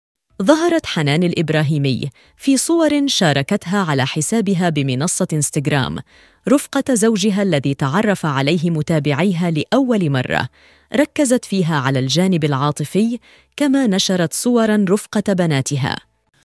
الفنانة المغربية حنان الإبراهيمي تفاجئ متابعيها بـ صور رومانسية رفقة زوجها وبناتها hanane ibrahimi
الفنانة المغربية حنان الإبراهيمي تفاجئ متابعيها بـ صور رومانسية رفقة زوجها وبناتها hanane ibrahimi Dounia batma ibtissam batma Mohamed turk